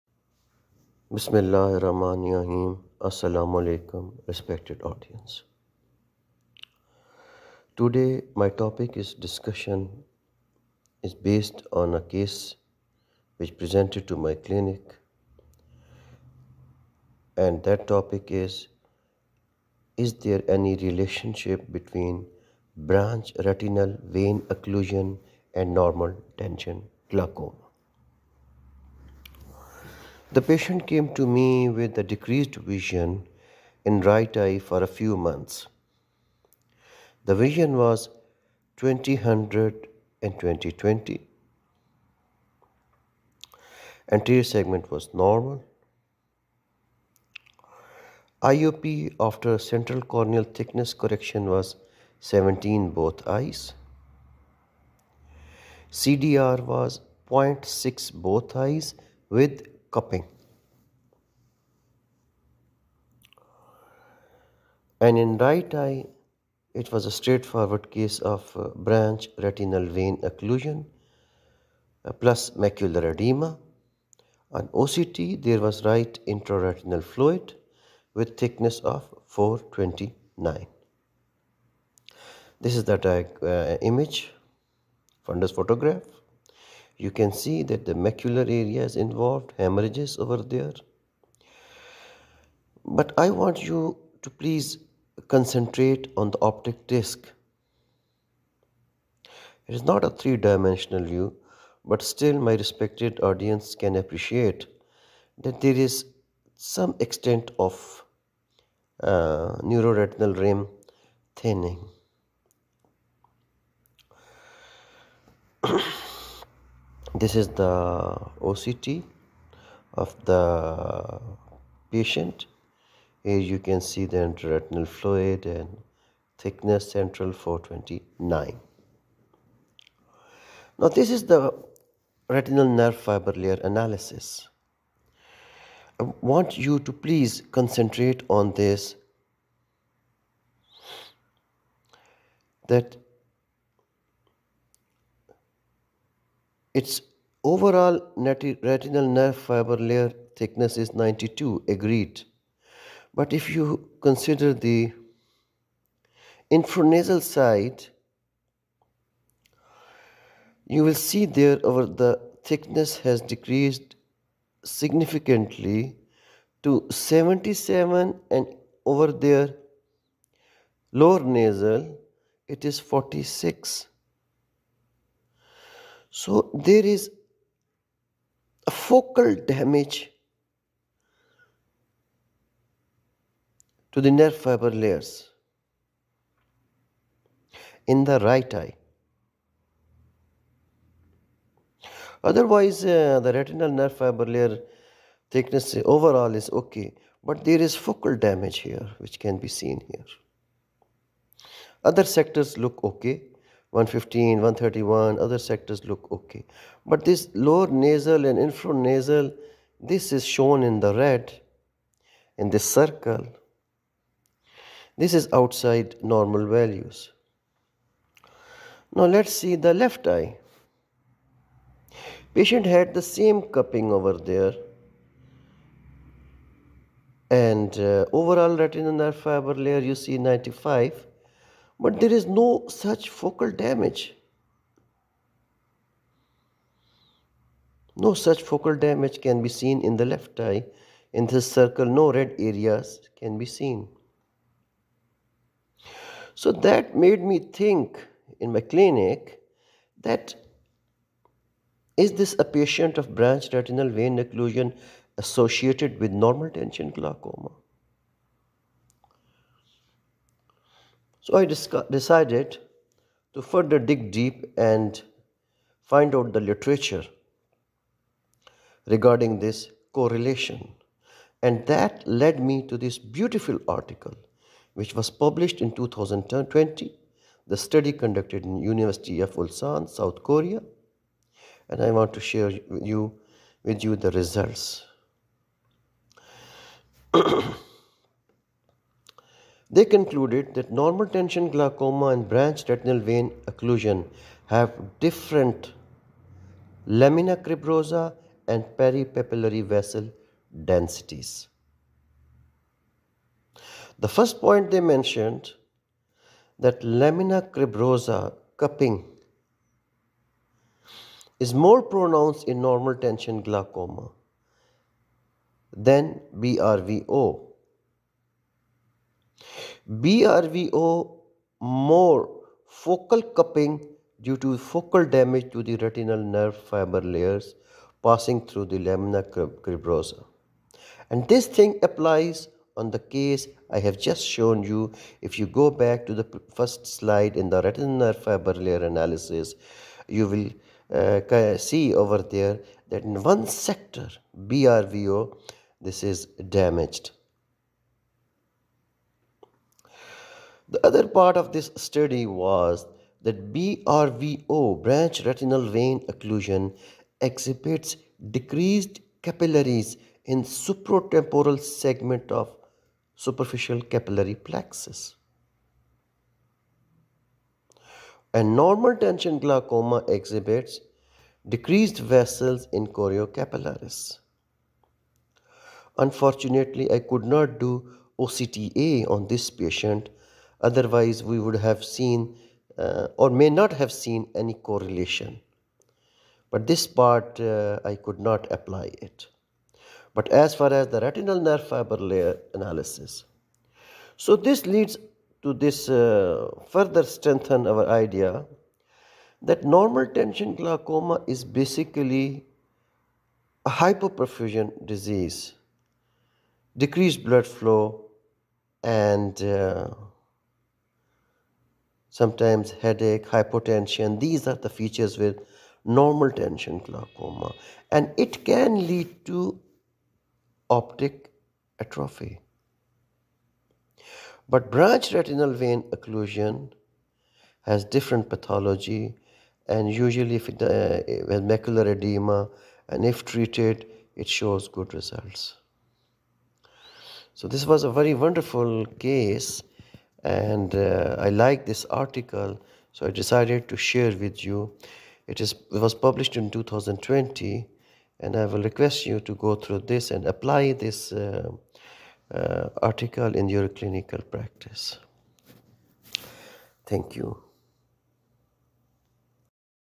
BRANCH RETINAL VEIN OCCLUSION AND NORMAL TENSION GLAUCOMA
BRVO AND NTG HAVE DIFFERENT PATHOLOGY